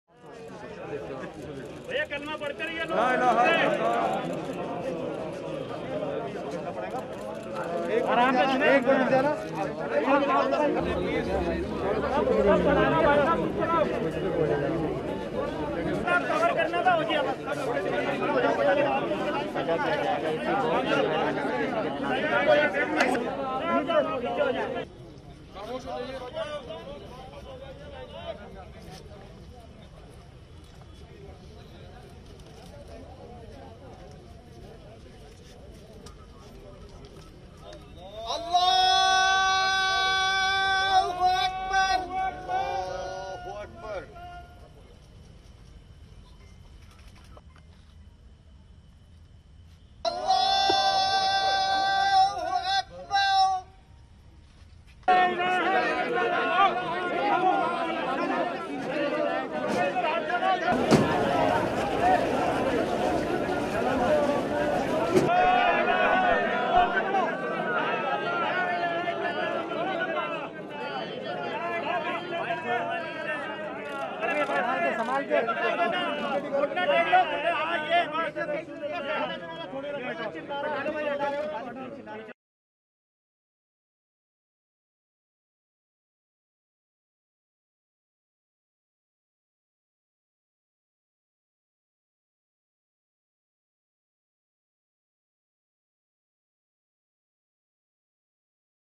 Body of slain journalist Danish Siddiqui returned from Afghanistan for burial in New Delhi
Slain Reuters journalist Danish Siddiqui was buried on July 18, 2021, in his Indian hometown of New Delhi. He was killed in Afghanistan on July 16 while covering clashes between Afghan security forces and the Taliban. Hundreds of friends, family and colleagues attended the funeral of the late journalist, who is survived by his wife and two children.